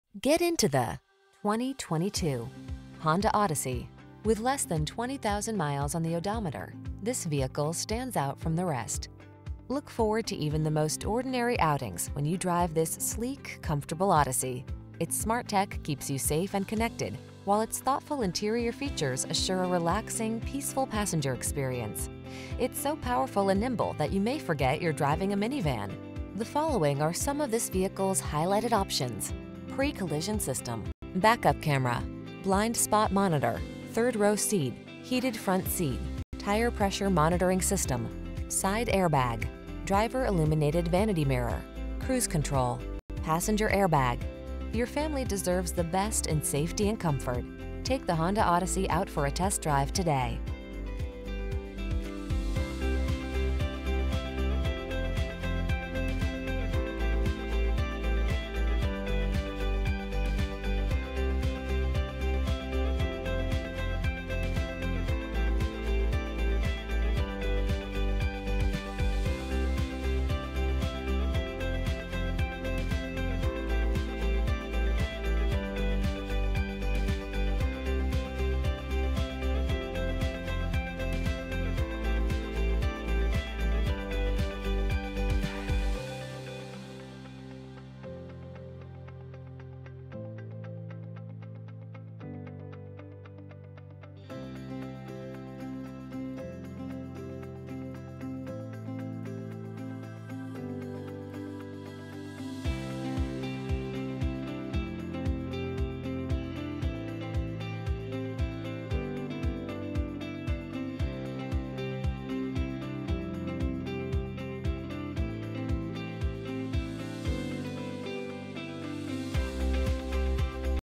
2022 Honda Odyssey EX AUTO Henderson, Las Vegas, Laughlin, St George, Flagstaff NV
Pre-Owned 2022 Honda Odyssey EX AUTO available at Findlay Honda Henderson located in 933 Auto Show Dr, Henderson, Nevada, 89014. Servicing the Henderson, Las Vegas, Laughlin, St George, Flagstaff area.

Get into the  2022  Honda Odyssey.  Look forward to even the most ordinary outings when you drive this sleek, comfortable Odyssey. Its smart tech keeps you safe and connected while its thoughtful interior features assure a relaxing, peaceful passenger experience. It's so powerful and nimble that you may forget you're driving a minivan! The following are some of this vehicles highlighted options: Pre-Collision System, Back-Up Camera, Blind Spot Monitor, 3rd Row Seat, Heated Front Seat(s), Tire Pressure Monitoring System, Side Air Bag, Driver Illuminated Vanity Mirror, Cruise Control, Passenger Air Bag. Your family deserves the best in safety and comfort. Take the Honda Odyssey out for a test drive today.